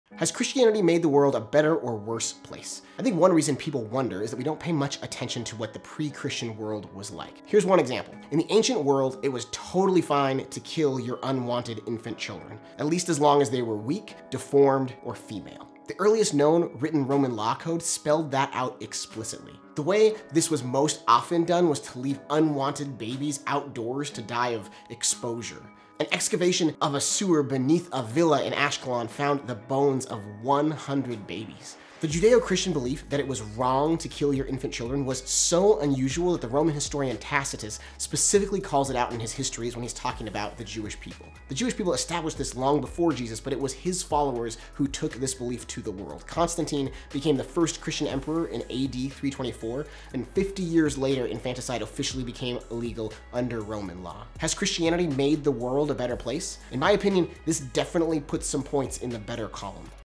Has Christianity made the world a better or worse place?
A lot of this comes from Rodney Stark’s book “The Rise of Christianity”. He’s an agnostic sociologist of religion and I really enjoyed both this book and “For the Glory of God: How Monotheism Led to Reformations, Science, Witch-Hunts, and the End of Slavery”. I’d highly recommend both.

“Tacitus charged that the Jewish teaching that it is “a deadly sin to kill an unwanted child” was but another of their “sinister and revolting” practices (The Histories 5.5, 1984 ed)”
The Rise of Christianity

The earliest Roman legal code that permitted killing any female infant and any weak or deformed male was called “The Twelve Tables”

The Twelve Tables—the earliest known Roman legal code, written about 450 BCE—permitted a father to expose any female infant and any deformed or weak male infant (Gorman 1982:25)

I think this is also from “The Rise of Christianity”, but I forgot to write it down. Fortunately it includes its own citation of its source material.
“During recent excavations of a villa in the port city of Ashkelon, Lawrence E. Stager and his colleagues made a gruesome discovery in the sewer that ran under the bathhouse.
‘The sewer had been clogged with refuse sometime in the sixth century A.D. When we excavated and dry-sieved the desiccated sewage, we found [the] bones… of nearly 100 little babies apparently murdered and thrown into the sewer. (1991:47) Examination of the bones revealed them to be newborns, probably day-olds (Smith and Kahila 1991).

Welcome back to the Thinking Believer  -- a YouTube channel taking a reasonable approach to the Christian faith.